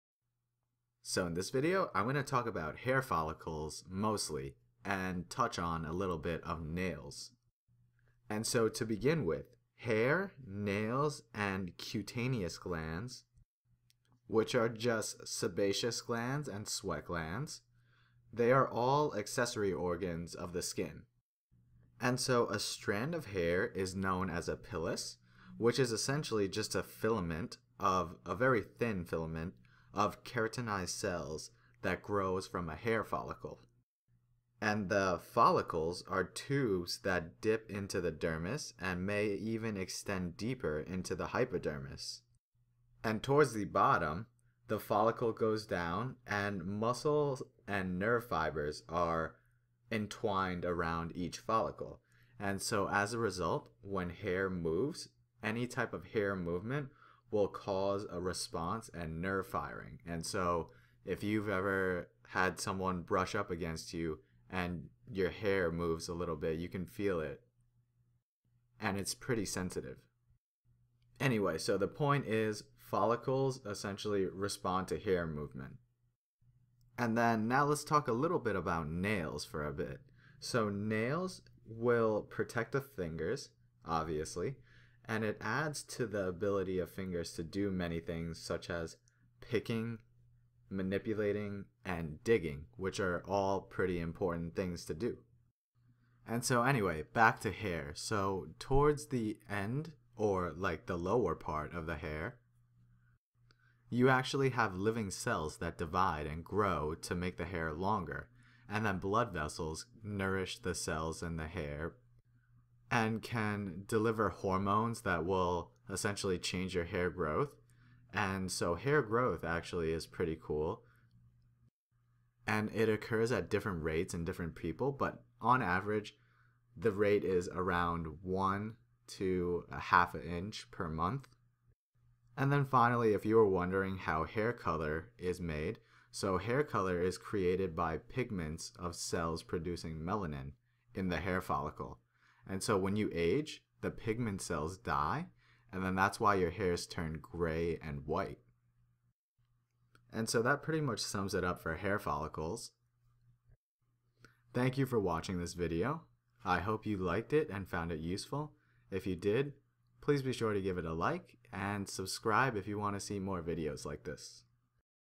Hair Follicles and Hair Introduction
This video explains the anatomy of hair follicles and introduces hair